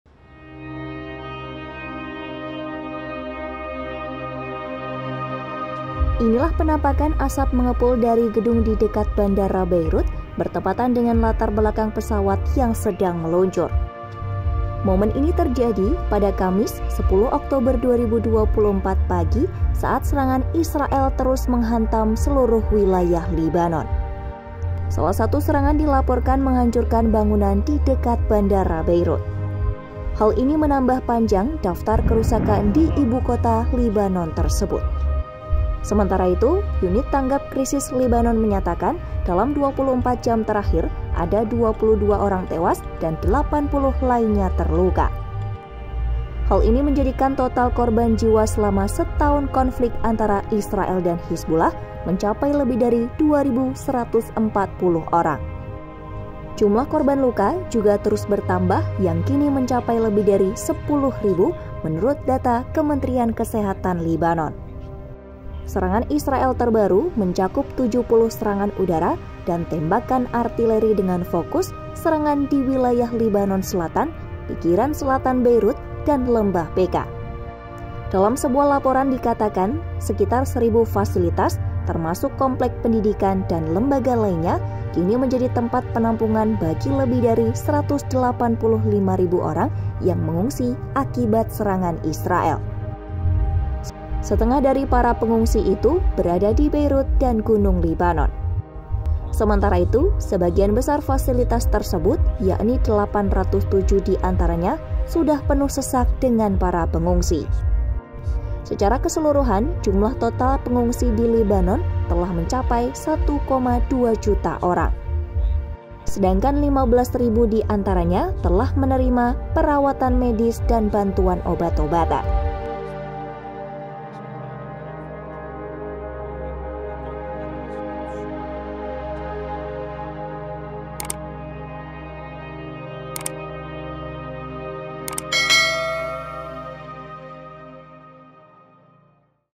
Penampakan Asap Hitam di Dekat Bandara Beirut Saat Israel Terus Lancarkan Serangan
Asap tebal terlihat membumbung tinggi di atas langit Beirut pada Kamis (10/10/2024) pagi, saat serangan Israel terus menghantam berbagai wilayah di Lebanon.

Salah satu serangan yang dilaporkan menghancurkan bangunan di dekat bandara Beirut, menambah panjang daftar kerusakan di ibu kota Lebanon tersebut.

Penulis Naskah: Novyana Nurmita Dewi
Narator: Novyana Nurmita Dewi
Video Editor: Agung Setiawan
Produser: Dandy Bayu Bramasta